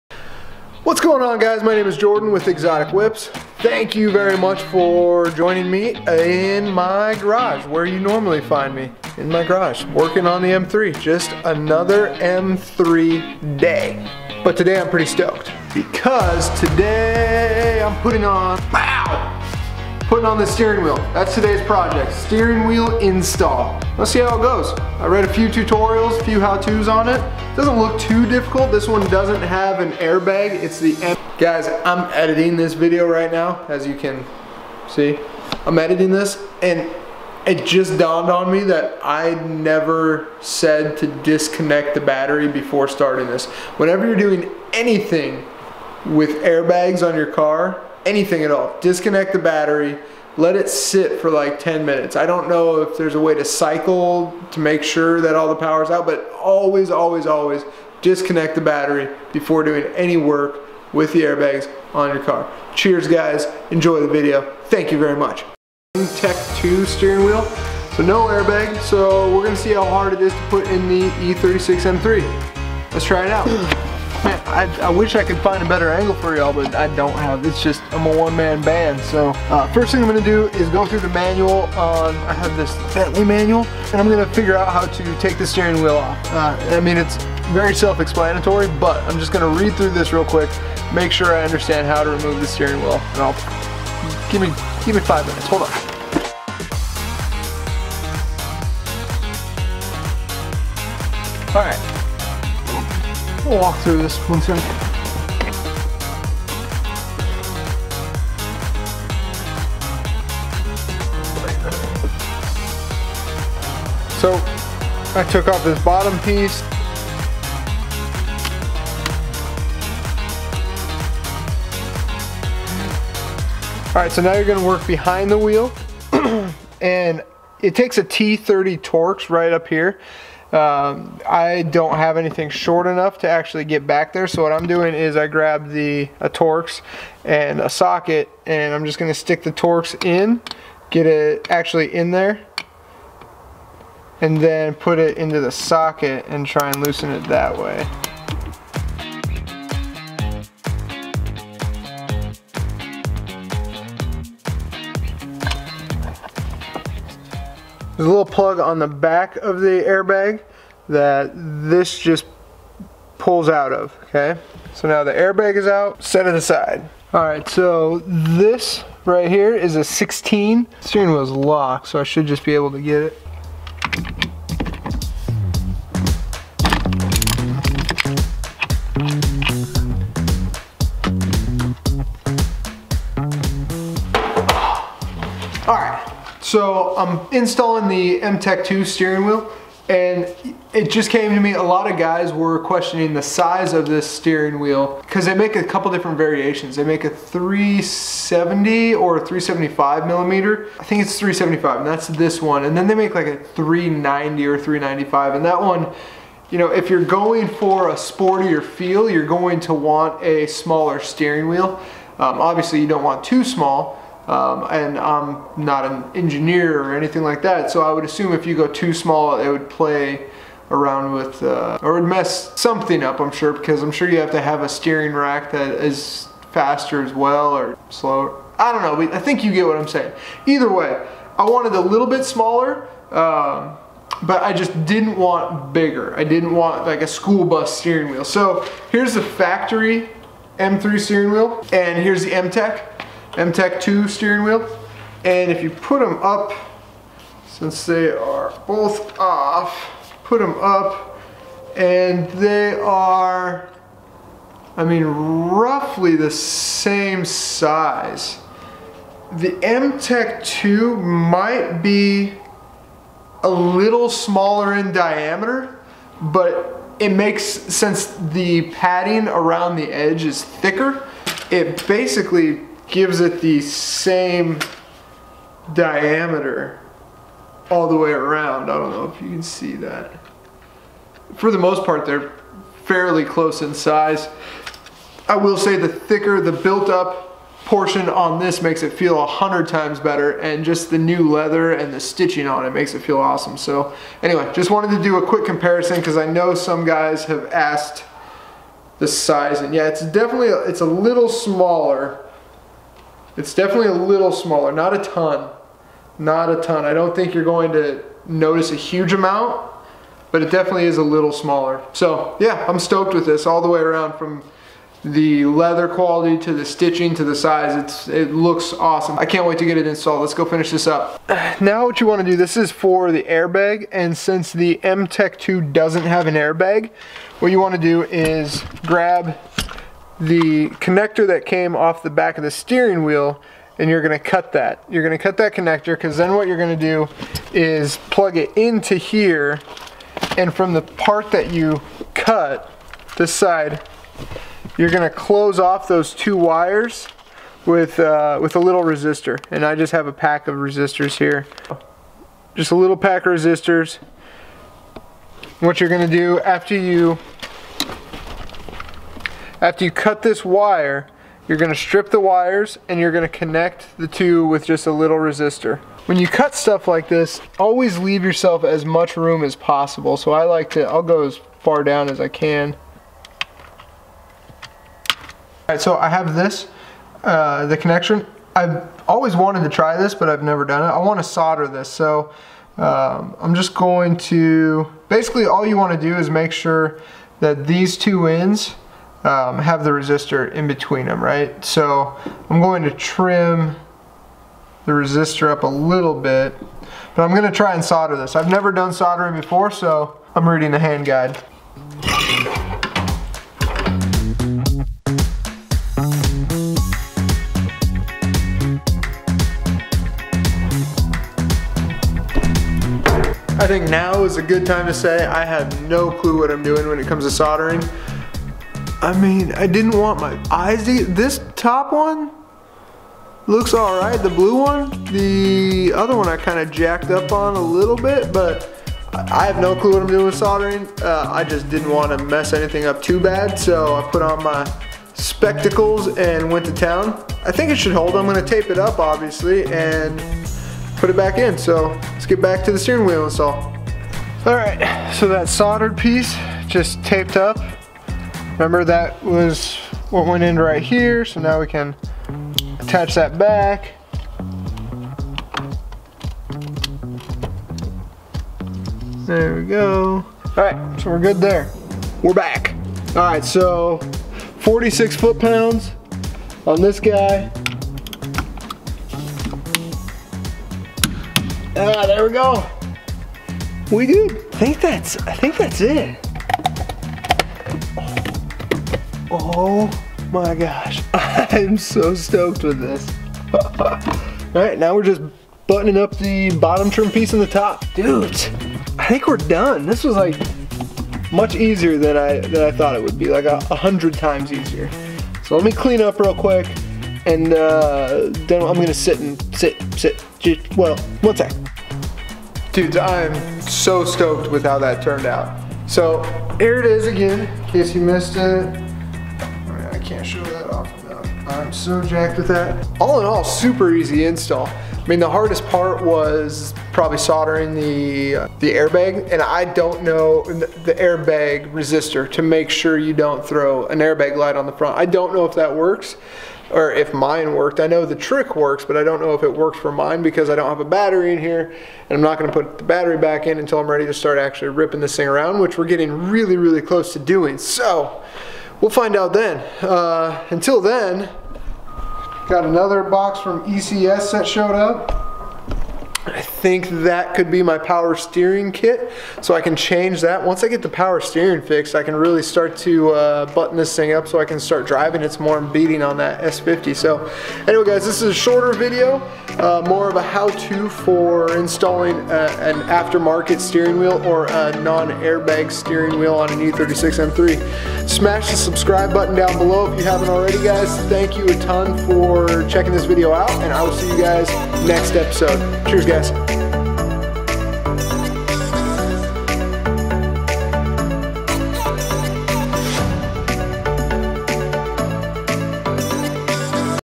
E36 M3 M-Tech 2 Steering Wheel Install with No Airbag Warning Light
Finally got around to installing an M-Tech 2 steering wheel on my E36 M3. This steering wheel doesn't have an airbag so I had to use a 1/2 watt 3.3 ohm resistor.

Signup to the new forum! MForum.Net